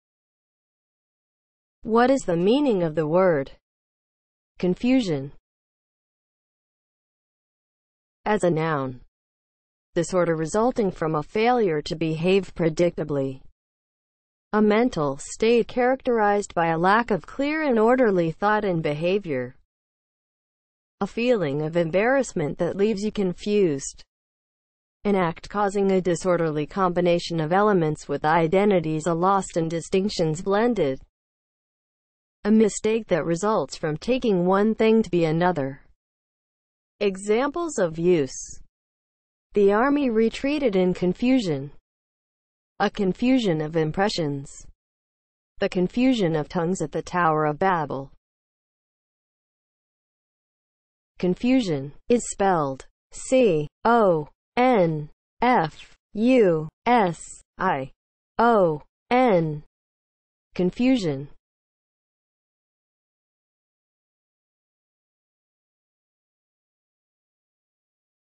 What is the meaning of the word CONFUSION?
Definition, examples of use and spelling of the word CONFUSION.

(00:00) Intro
(00:07) As a noun
(00:49) Spelling

As a noun (00:07):
• disorder resulting from a failure to behave predictably
• a mental state characterized by a lack of clear and orderly thought and behavior
• a feeling of embarrassment that leaves you confused
• an act causing a disorderly combination of elements with identities lost and distinctions
  blended
• a mistake that results from taking one thing to be another

Examples:
• "the army retreated in confusion"
• "a confusion of impressions"
• "the confusion of tongues at the Tower of Babel"